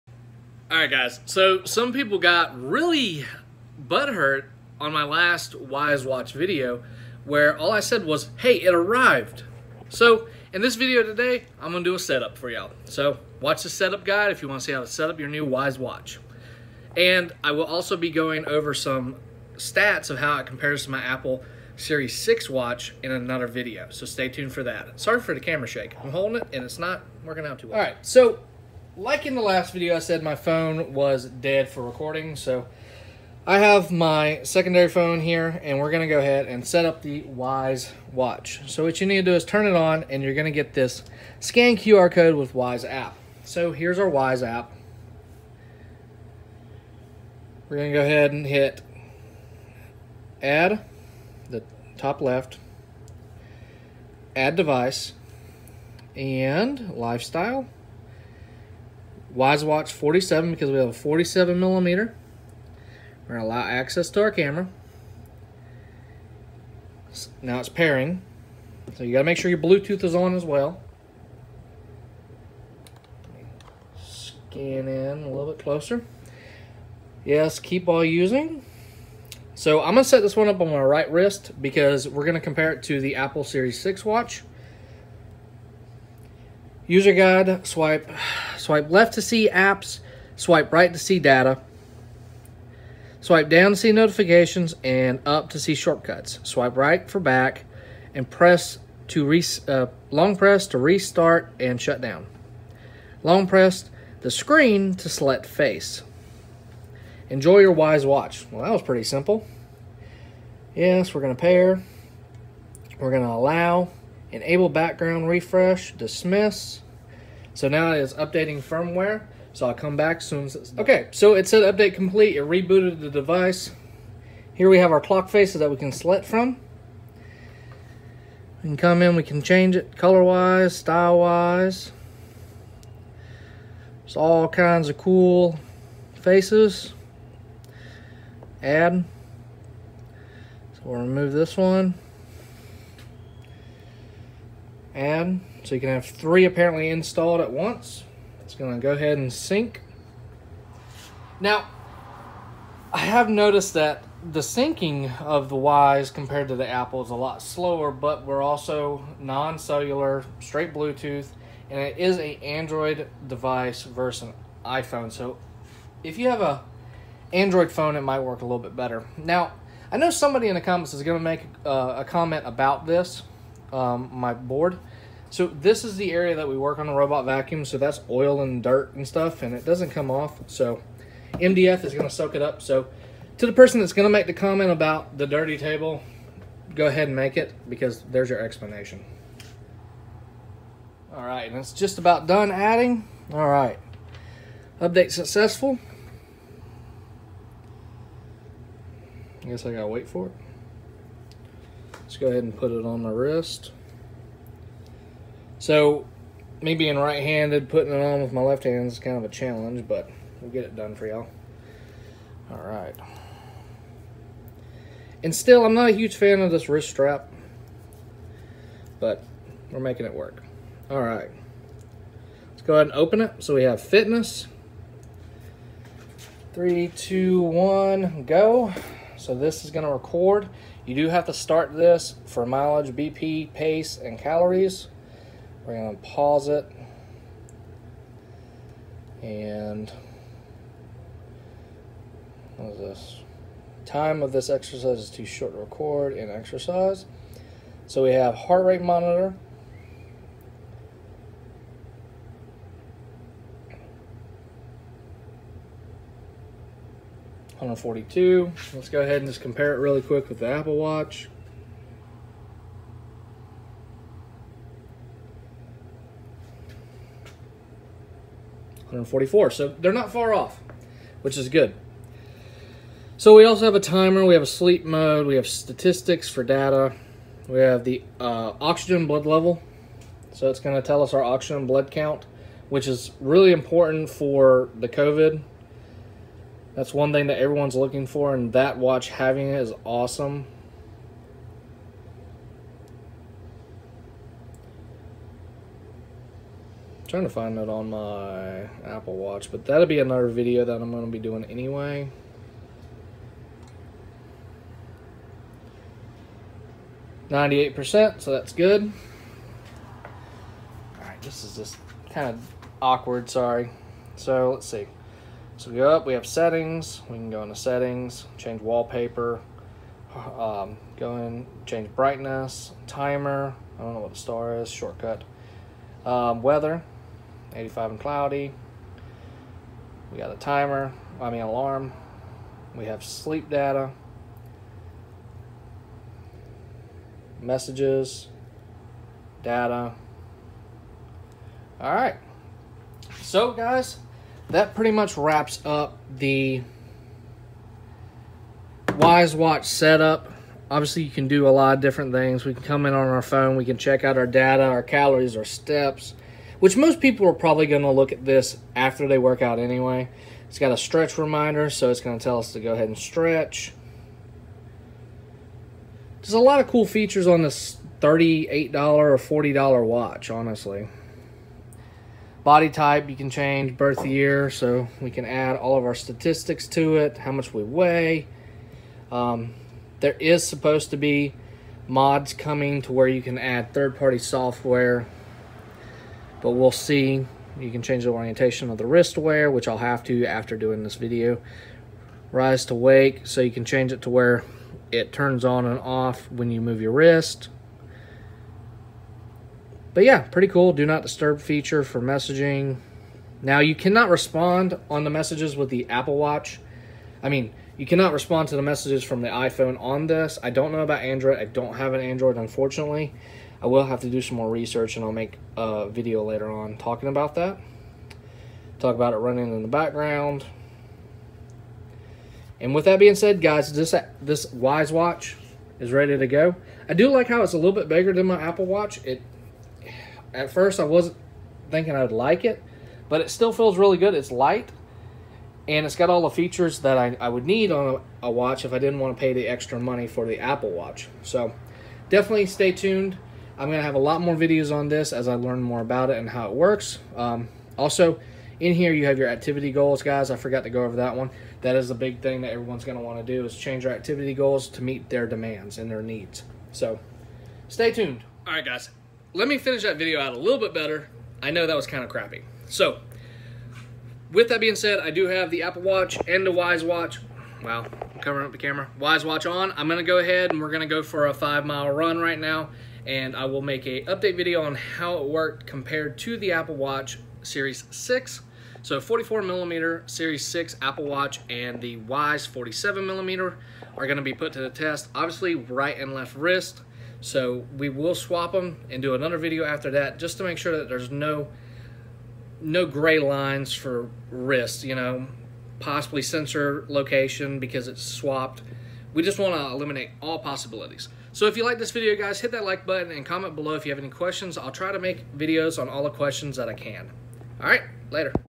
Wyze Smart Watch 47 setup guide
In today’s video we will be setting up a wyze smart watch for the first time super simple and easy.